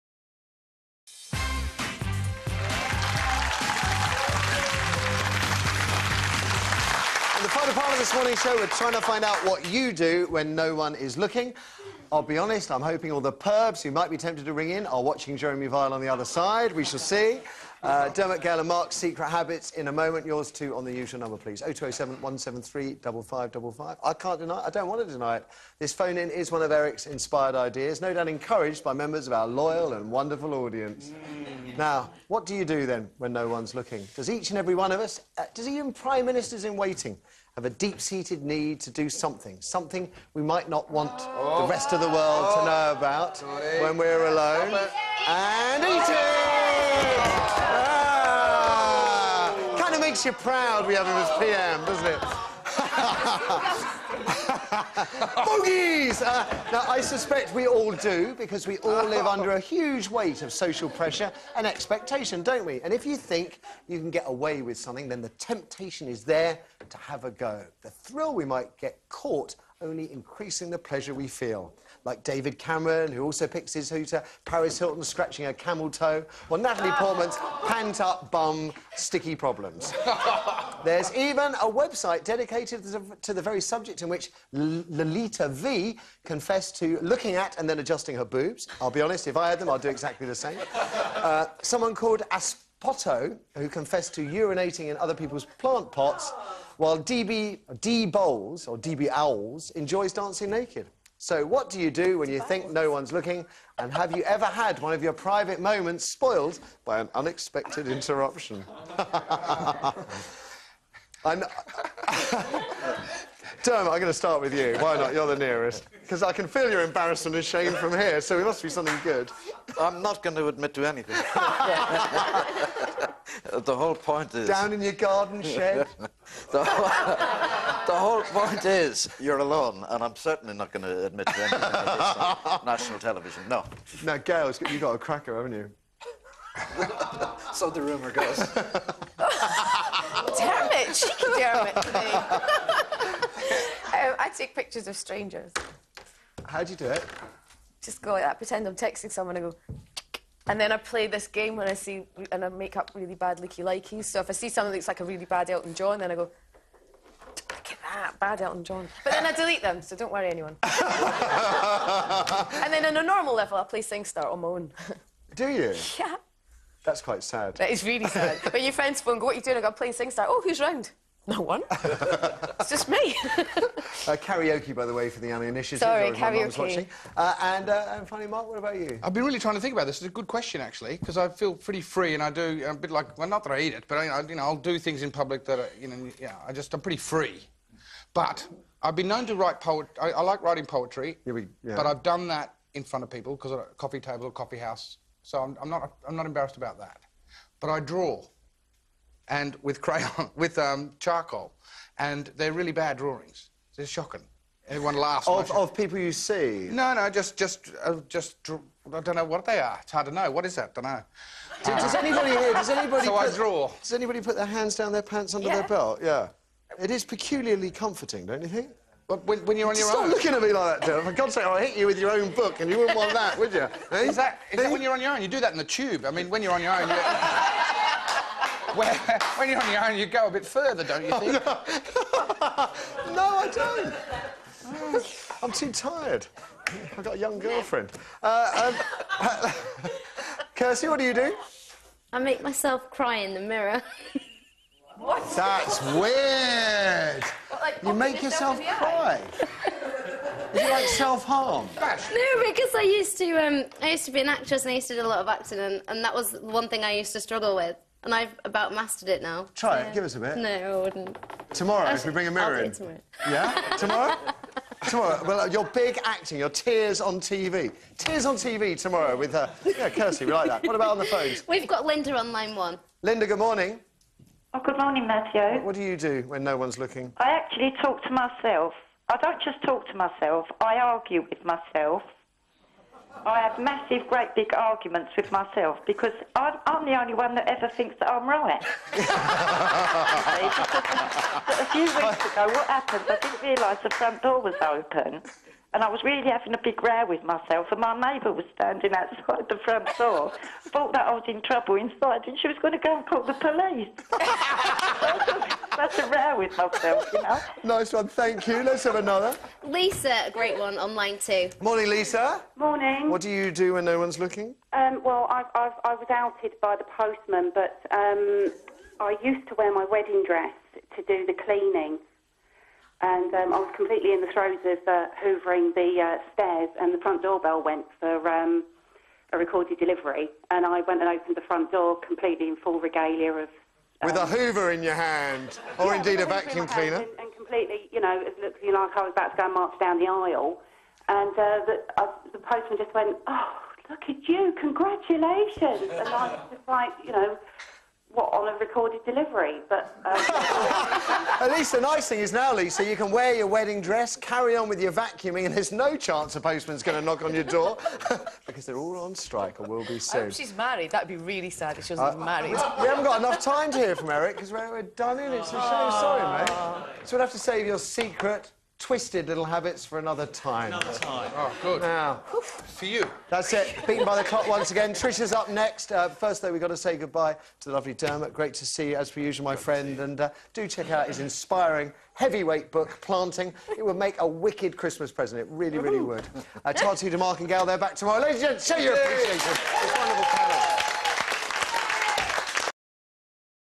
What do you do when no-one's looking? (14.10.09) - TWStuff
Diarmud Gavin appeared on the show to promote his new book, Planting.